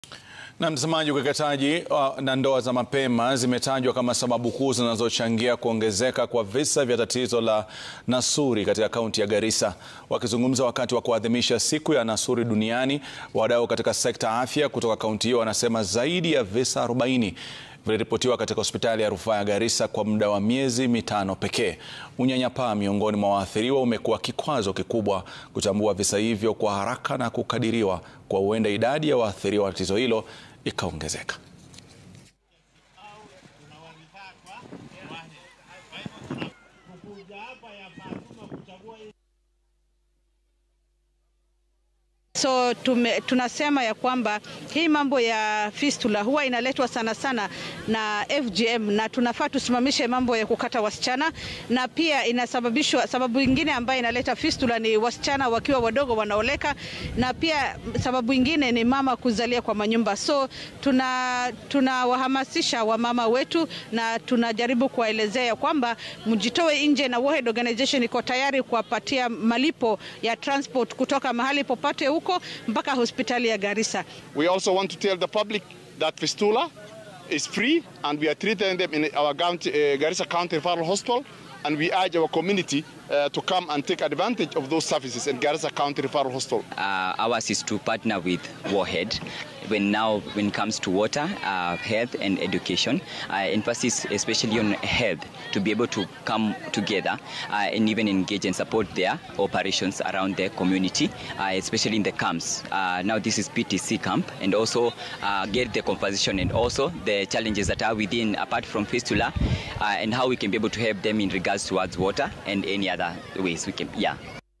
Ukeketaji na ndoa za mapema zatajwa kama sababu zinazochangia kuongezeka kwa visa vya tatizo la nasu
Wakizungumza wakati wa kuadhimisha siku ya nasuri duniani, wadau katika sekta ya afya kutoka kaunti hiyo wanasema zaidi visa 40 viliripotiwa katika hospitali ya rufaa ya Garissa kwa muda wa miezi tano pekee. Unyanyapaa miongoni mwa waathiriwa umekuwa kikwazo kikubwa kutambua visa hivyo kwa haraka na kukadiriwa kuwa huenda idadi ya waathiriwa wa tatizo hilo ikaongezeka.